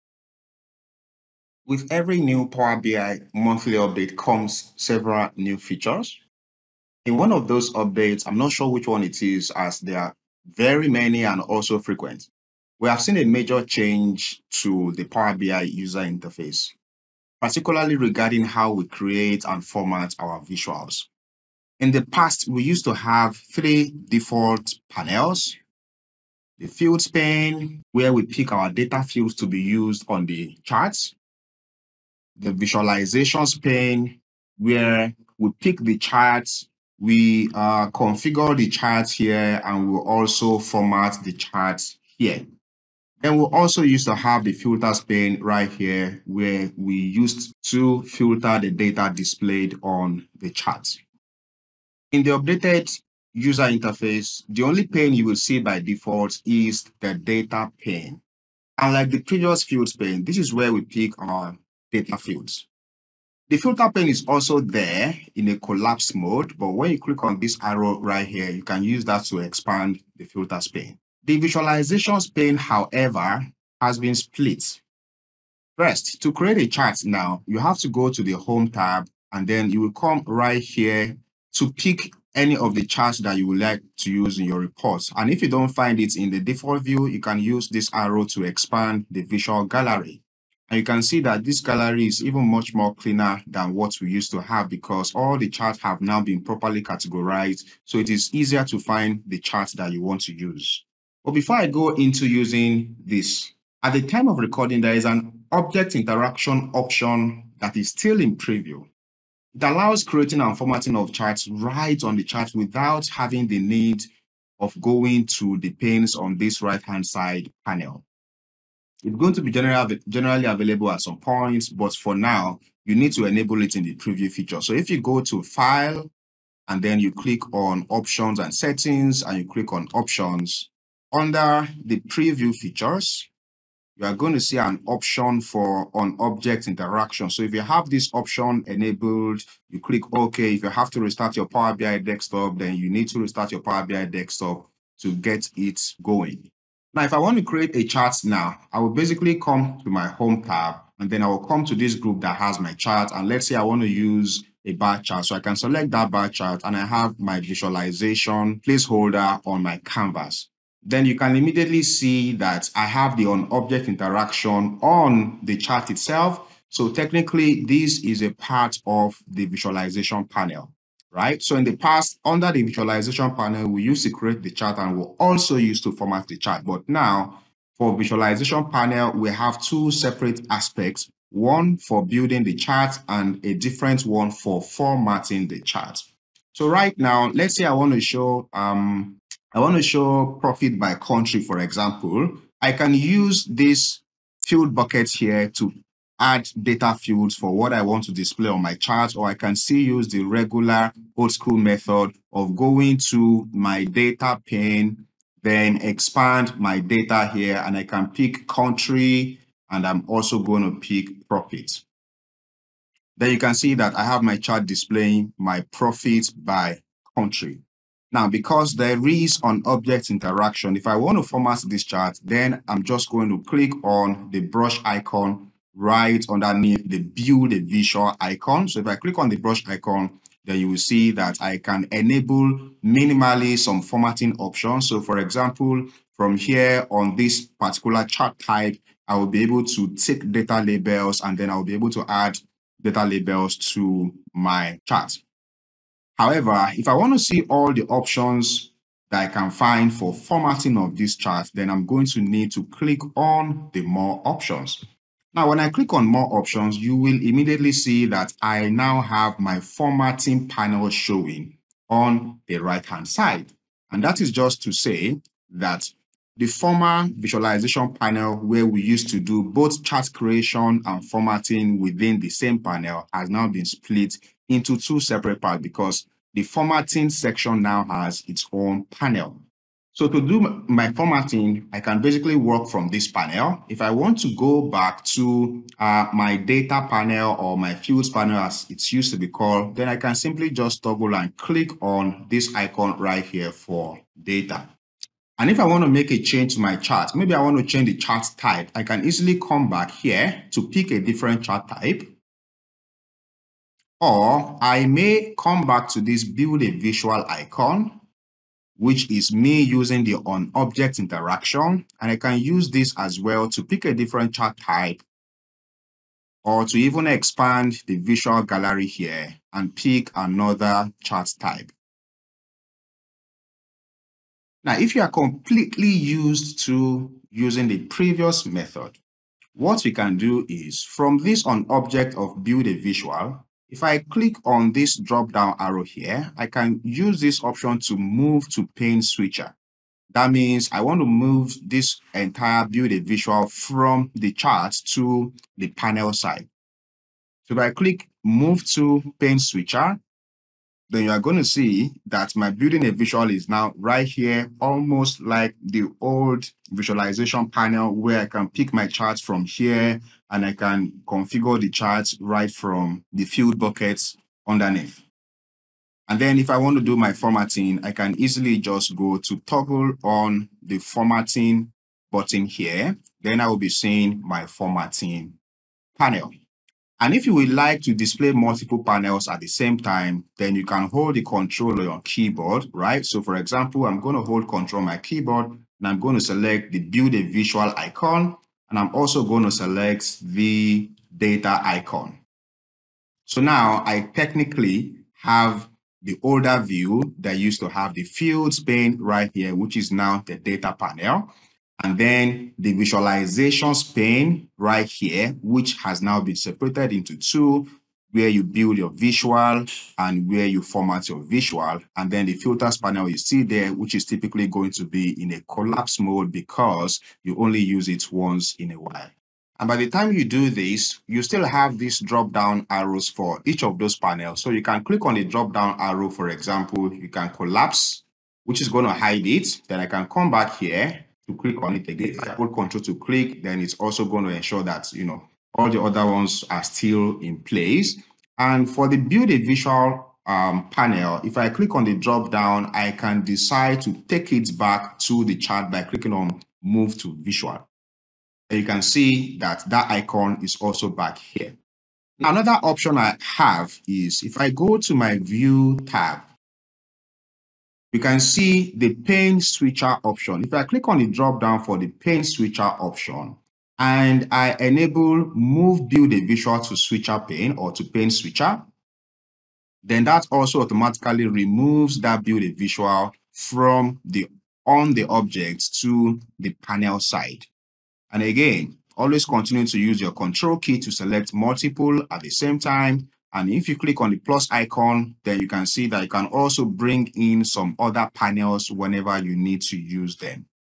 How to create Power BI Charts after the new UI update
In this video, Ahmed Oyelowo describes how to create Power BI Charts after the changes made to the Power BI User Interface.